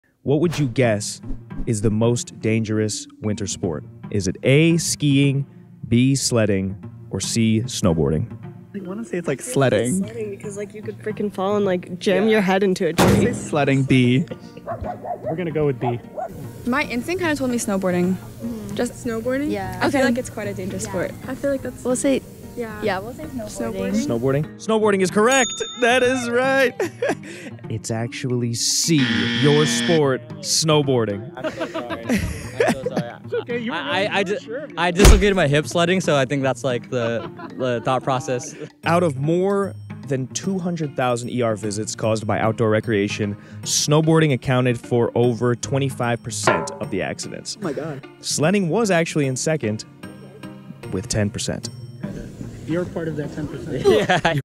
"YOU WERE PART OF THAT 10%"
Join us as we quiz the people of Los Angeles on common holiday misconceptions. Who will get this surprise pop quiz right?